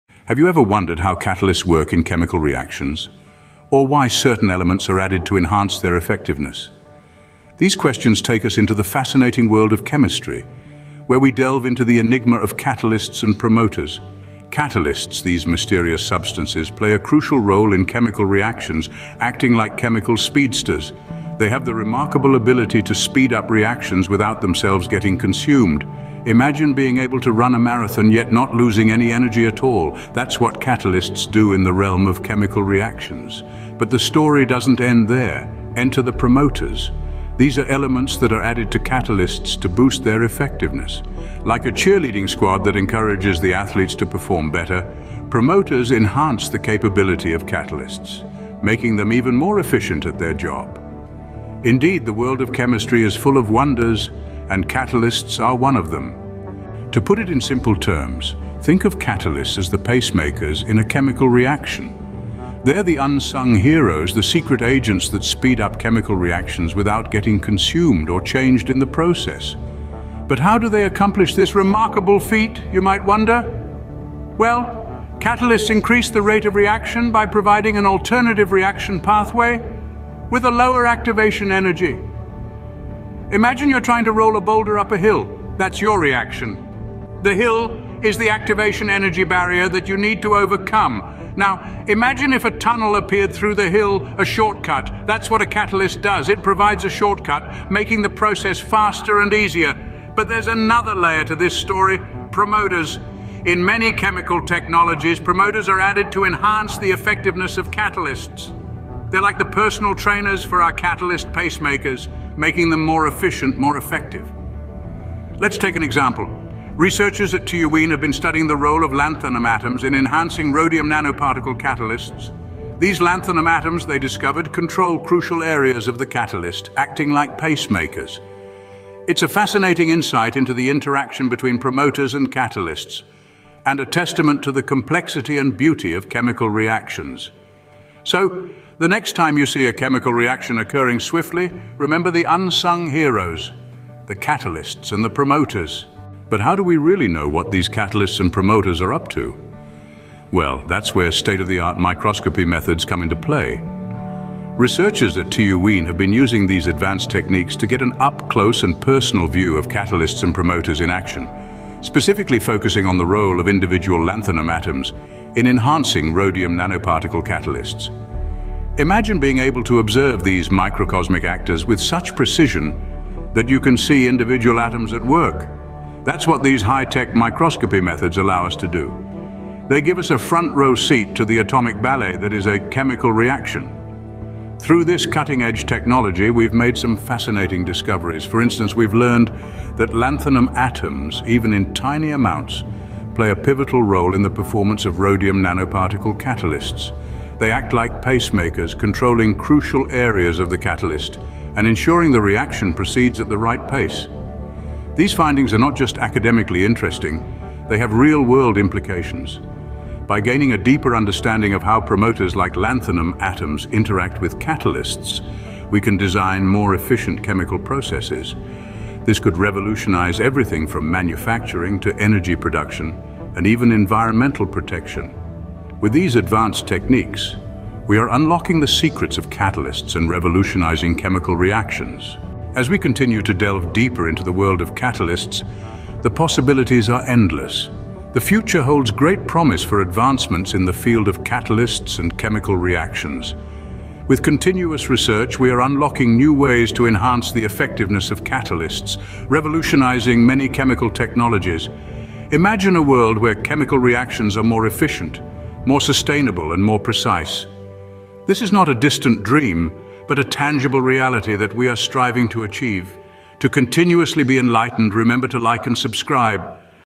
Unlocking Catalyst Secrets: A Revolutionary Breakthrough
In this captivating video, join us as we delve into the fascinating world of catalyst promoters! Discover the groundbreaking research conducted by the brilliant minds at TU Wien, as they unravel the secrets behind these catalyst enhancers. Get ready to be amazed by their revolutionary findings and the potential impact they have on various industries. With in-depth explanations and visual representations, this scientific exploration promises to be both enlightening and engaging. Don't miss out on this incredible opportunity to expand your knowledge and be part of this scientific breakthrough.